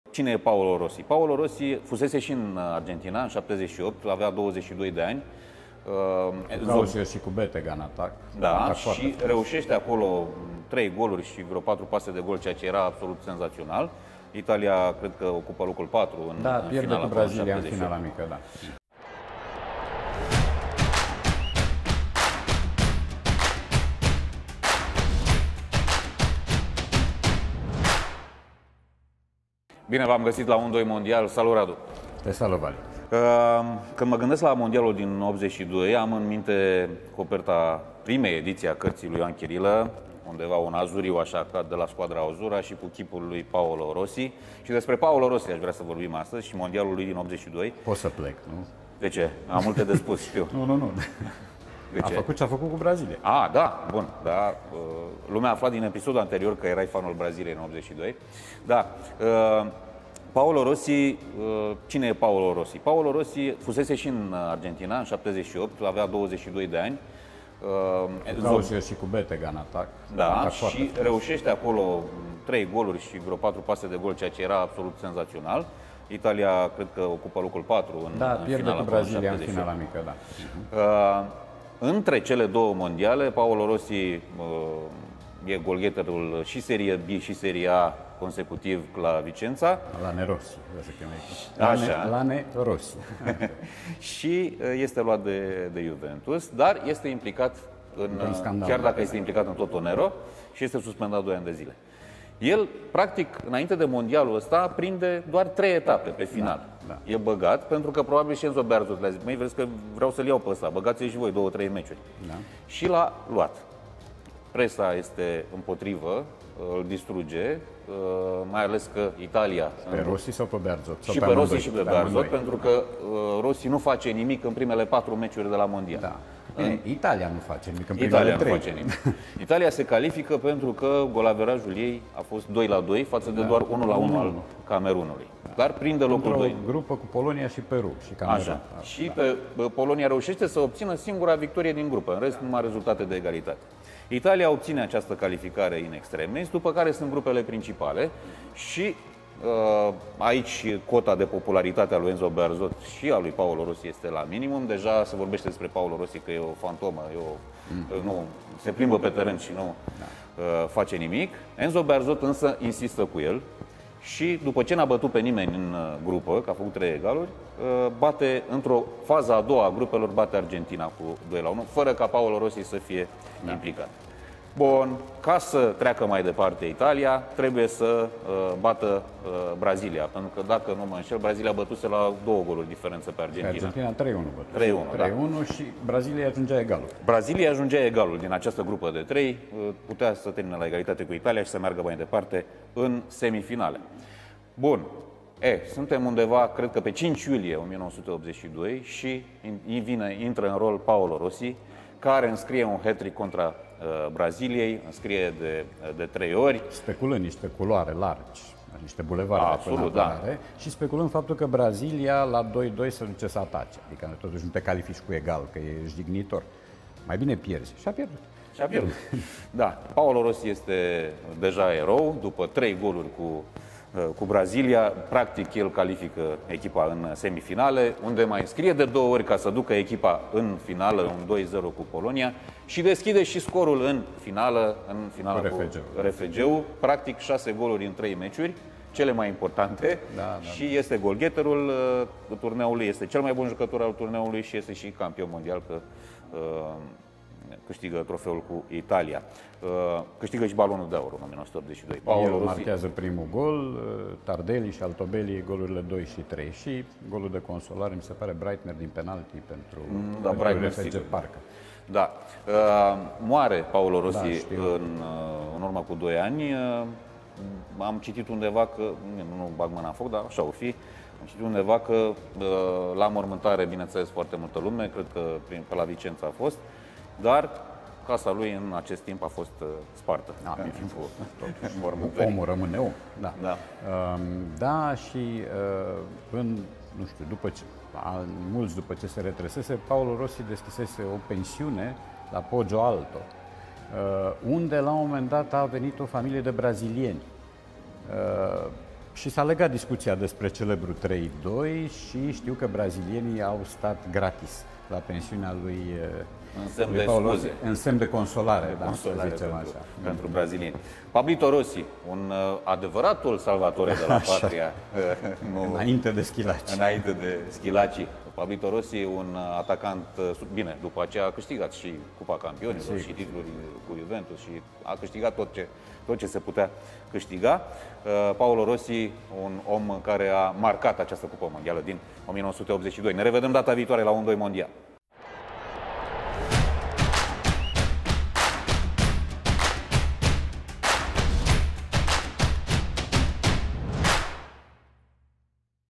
Vara de poveste a lui "Pablito". Un-Doi Mondial, cu Radu Paraschivescu și Vali Moraru
La Cupa Mondială din 1982, Paolo Rossi a fost eroul Italiei. A făcut Brazilia să plângă, și-a reabilitat numele și a fost golgheterul turneului câștigat de Italia.

Radu Paraschivescu își amintește de Mundialul la care Paolo a devenit "Pablito".